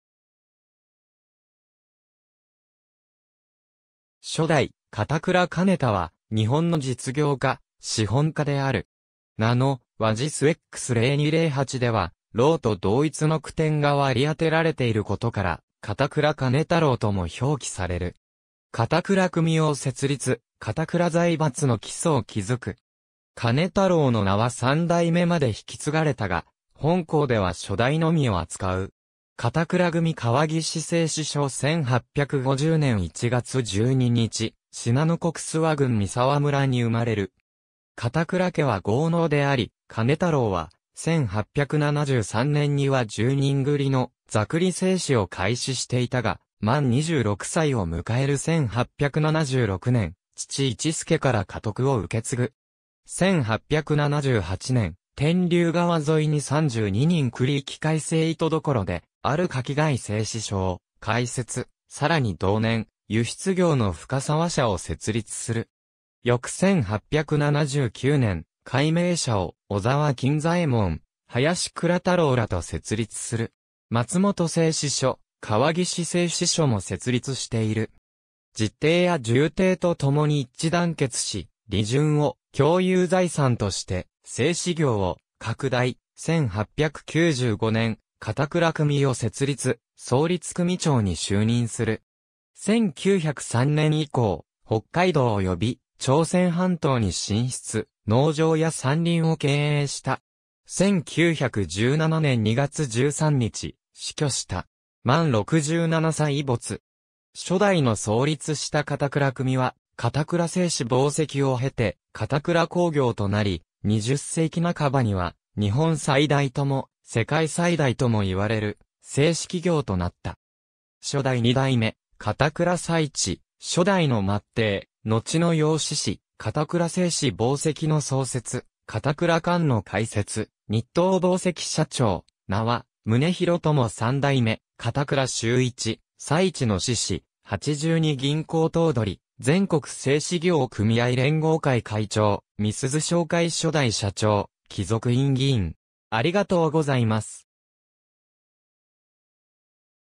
片倉兼太郎
初代 片倉 兼太郞（しょだい かたくら かねたろう、嘉永2年11月29日 / グレゴリオ暦 : 1850年1月12日 - 1917年2月13日）は、日本の実業家、資本家である。
名の「郞」はJIS X 0208では「郎」と同一の区点が割り当てられていることから、片倉 兼太郎（かたくら かねたろう）とも表記される。
片倉組（現在の片倉工業）を設立、片倉財閥の基礎を築く。
兼太郎の名は3代目まで引き継がれたが、本稿では初代のみを扱う。
片倉組川岸製糸所 1850年1月12日（嘉永2年11月29日）、信濃国諏訪郡三沢村（のちの川岸村、現在の長野県岡谷市川岸）に生まれる。
片倉家は豪農であり、兼太郎は、1873年（明治6年）には10人繰りの座繰製糸を開始していたが、満26歳を迎える1876年（明治9年）、父市助（いちすけ）から家督を受け継ぐ。
1878年（明治11年）、天竜川沿いに32人繰り器械製糸所である垣外製糸所を開設、さらに同年、輸出業の深沢社を設立する。
翌1879年（明治12年）、開明社を尾澤金左衞門、林倉太郎らと設立する。
松本製糸所、川岸製糸所も設立している。
実弟や従弟とともに一致団結し、利潤を共有財産として、製糸業を拡大、1895年（明治28年）、片倉組を設立、創立組長に就任する。
1903年（明治36年）以降、北海道および朝鮮半島に進出、農場や山林を経営した。
1917年（大正6年）2月13日、死去した。
満67歳没。
初代の創立した片倉組は、片倉製糸紡績（1920年）を経て片倉工業となり、20世紀半ばには、日本最大とも世界最大ともいわれる製糸企業となった。
初代 二代目 - 片倉 佐一（かたくら さいち、1862年 - 1934年）、初代の末弟（四男）、のちの養嗣子、片倉製糸紡績（現在の片倉工業）の創設、片倉館の開設、日東紡績社長、名は宗広（むねひろ）とも 三代目 - 片倉 脩一（かたくら しゅういち、1884年 - 1947年）、佐一の嗣子（長男）、八十二銀行頭取、全国製糸業組合連合会会長、美篶商会初代社長、貴族院議員